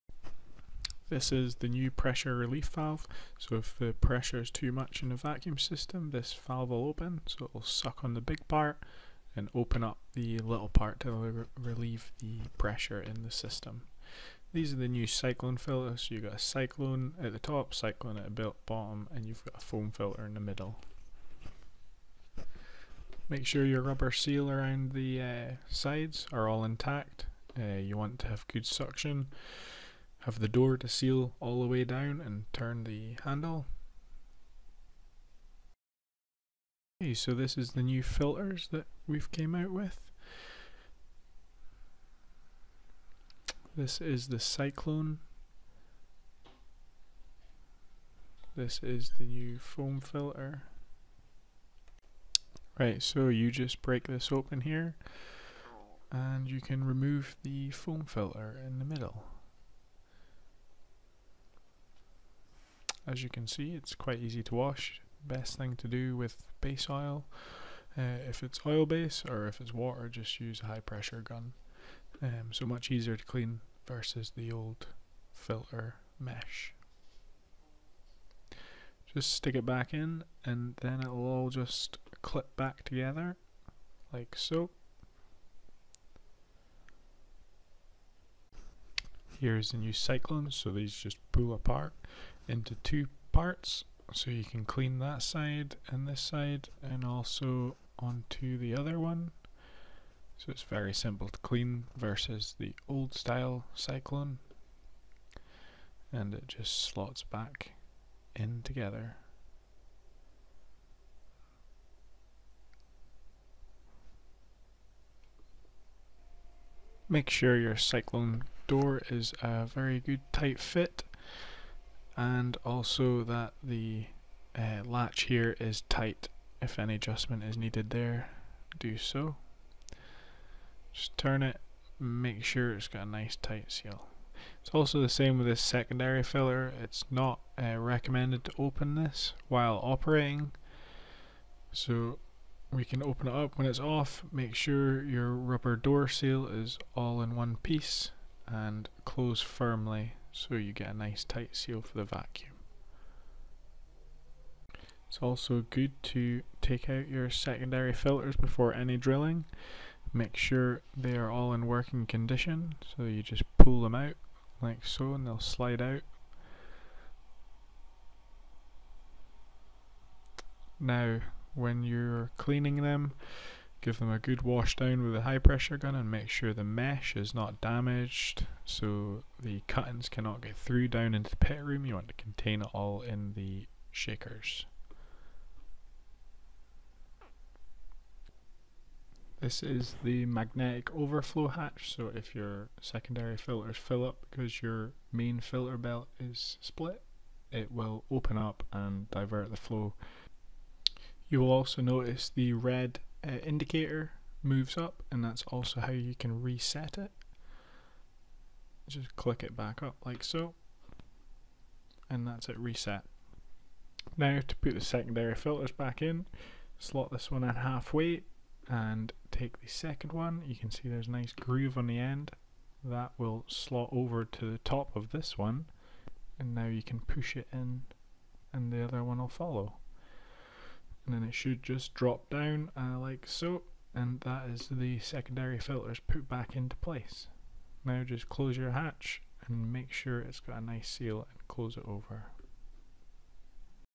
Vacuum filters and hatches on the MudCube
Overview of the vacuum filters and hatches on the MudCube